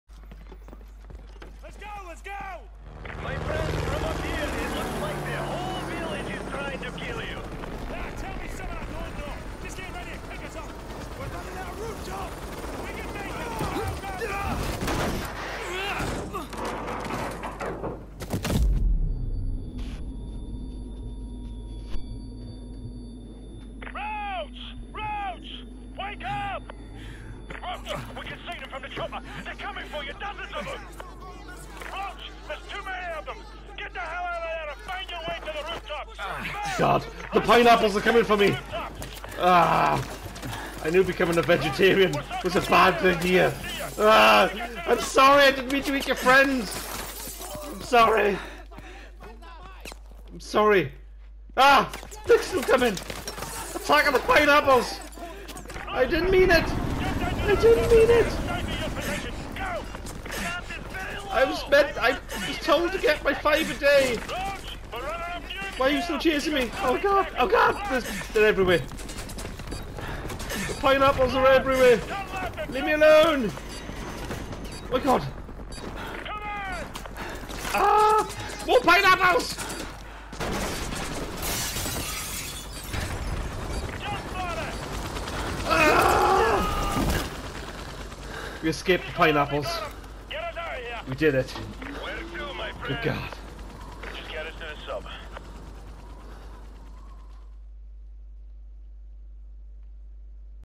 Attack Of The Killer Pineapples
Pineapples have become Sentient and are now trying to kill me Arghhh!!!